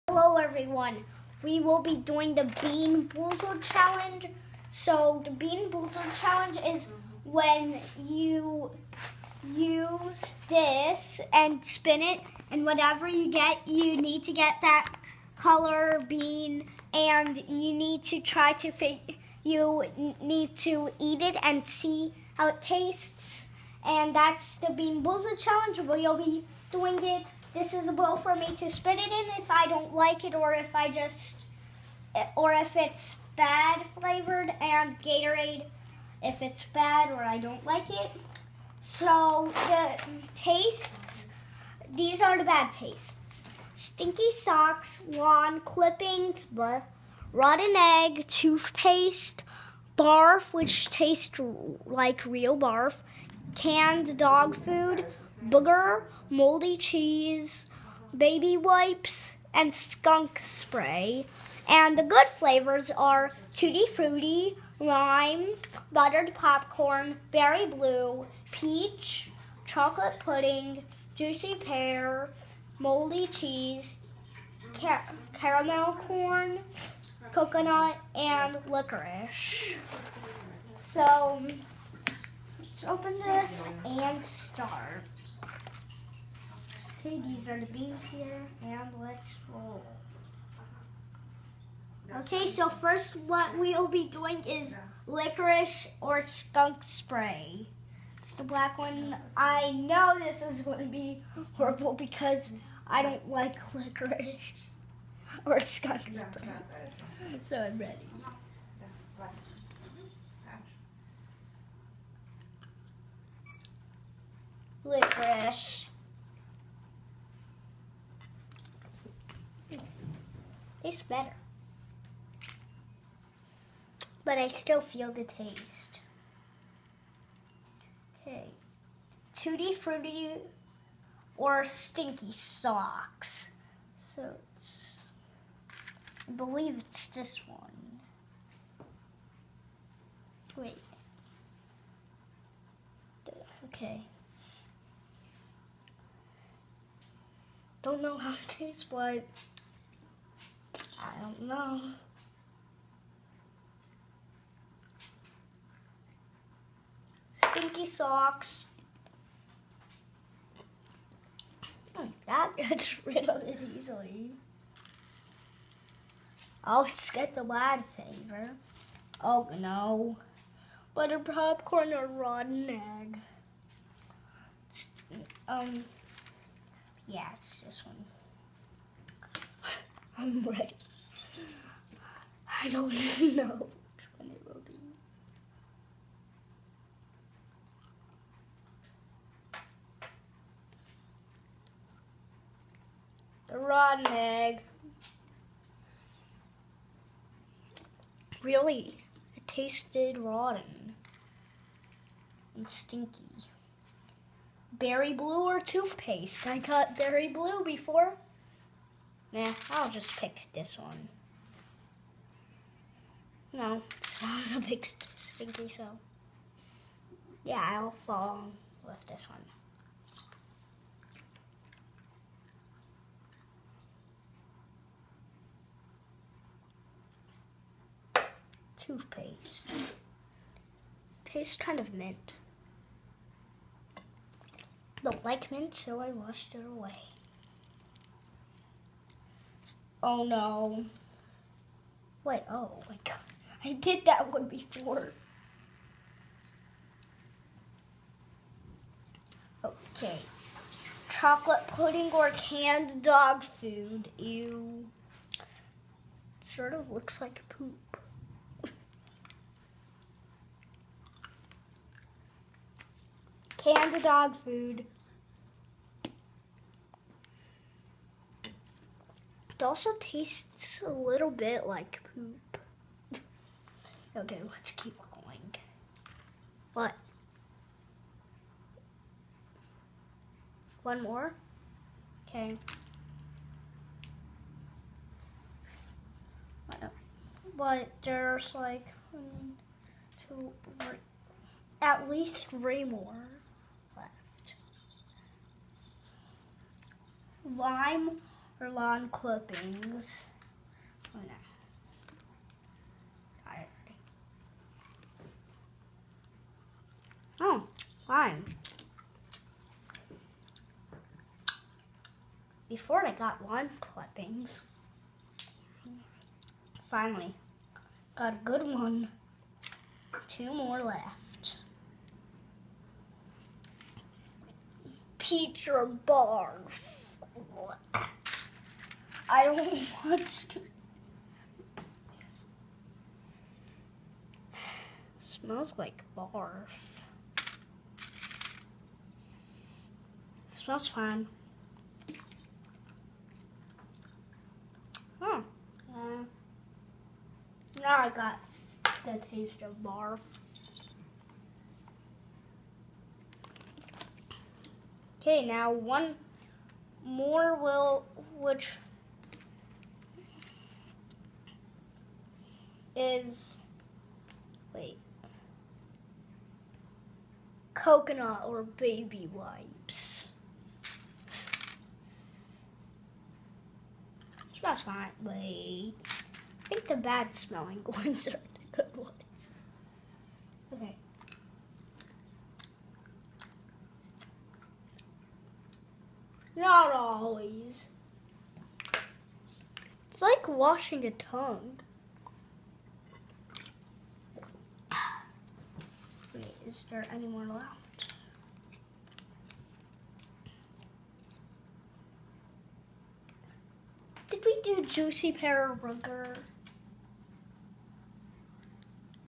Alex doing Bean Boozled Challenge
Alex doing Bean Boozled Challenge. This is a crazy game where you spin the pointer then you get to try a jelly bean there are good ones and bad ones including barf, boogers, and dog food